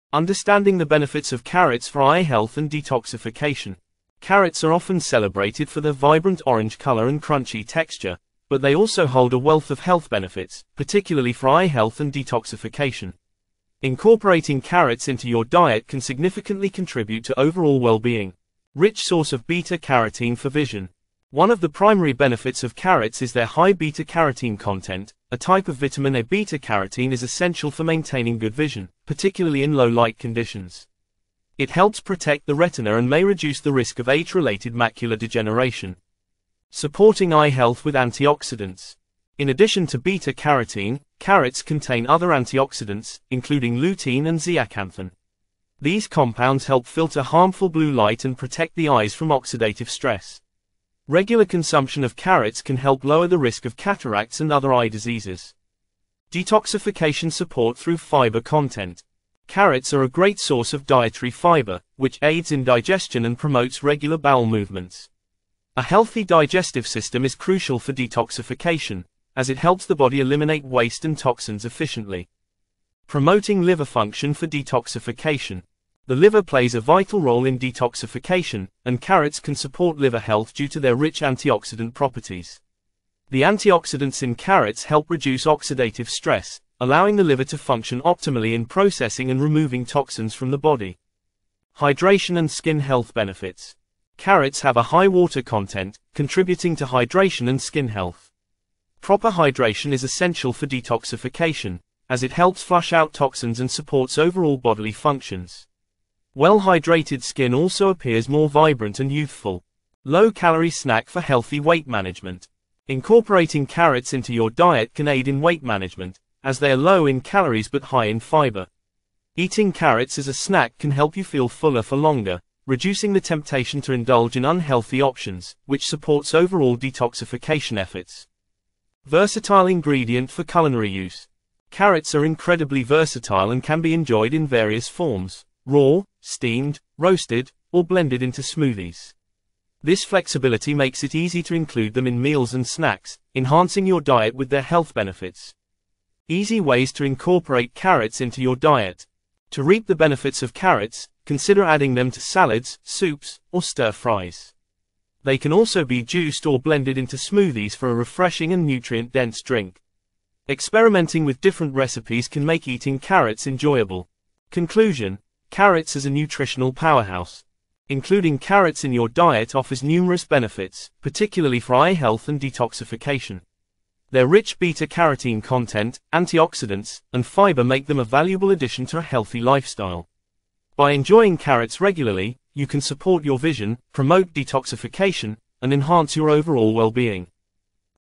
What Are the Benefits of Including Carrots for Eye Health and Detoxification
Carrots are known for supporting eye health and aiding detoxification. Learn why this vibrant vegetable should be part of your daily diet.